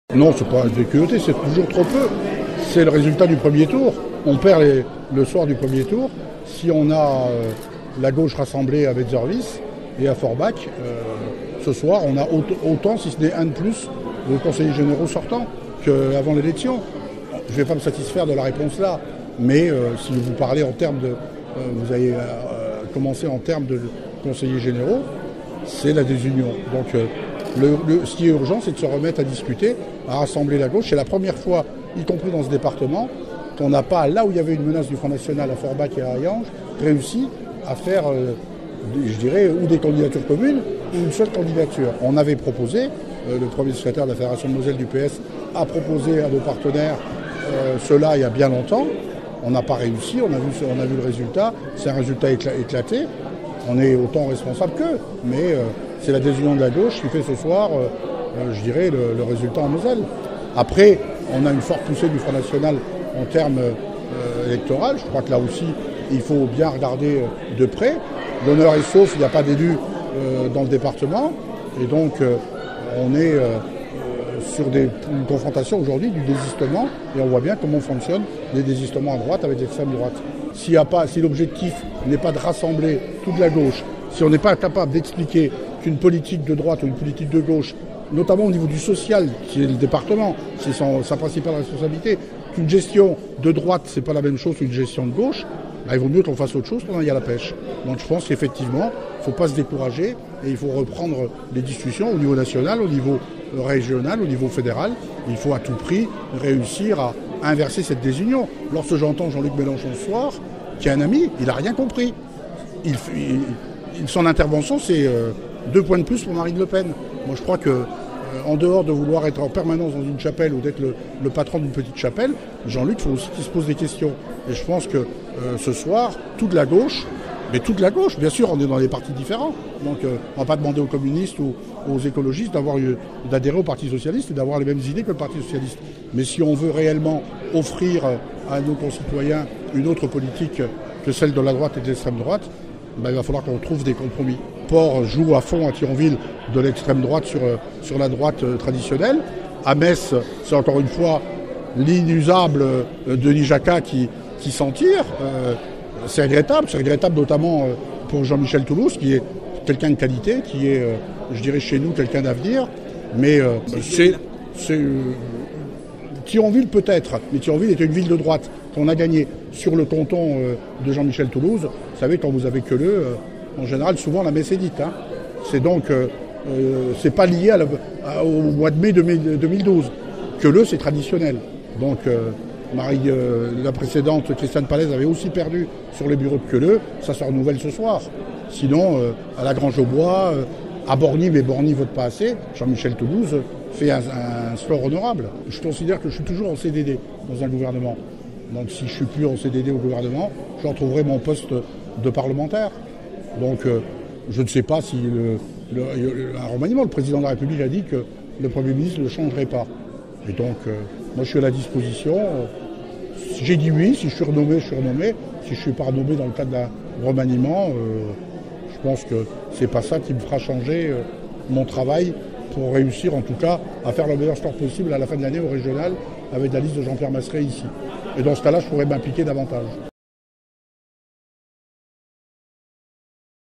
Moselle Élections départementales 2015 2e tour Jean-Marc Todeschini
Interview de Jean-Marc Todeschini Sénateur PS
secrétaire d'État chargé des Anciens Combattants et de la Mémoire, auprès du ministre de la Défense